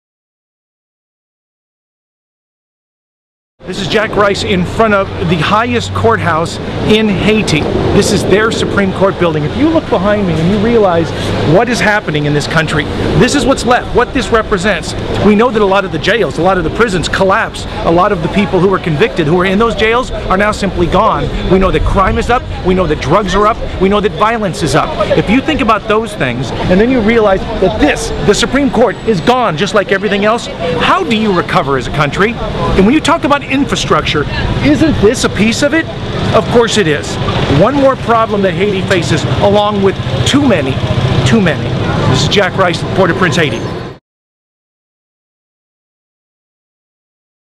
Justice Collapses in Haiti - So Do the Courts
Jack Rice reporting from in front of the Haitian Palais de Justice in Port-au-Prince, Haiti.  As infrastructure collapses, including justice, what happens next.